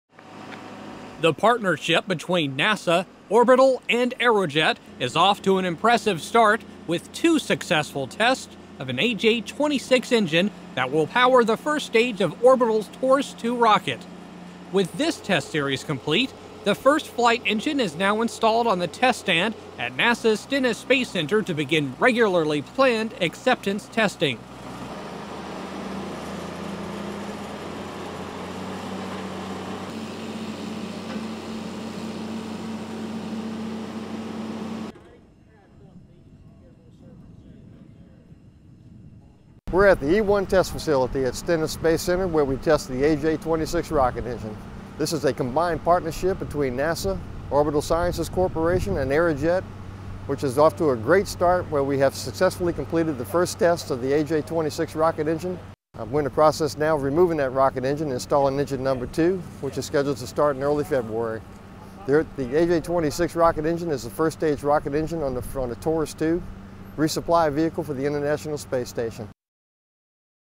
AJ26 Flight Engine Installed for Test Firing
The partnership between NASA, Orbital and Aerojet is off to an impressive start with two successful tests of an AJ26 engine that will power the first stage of Orbital's Taurus II rocket.  With this test series complete, the first flight engine is now installed on the test stand at NASA Stennis Space Center to begin regularly planned "acceptance testing."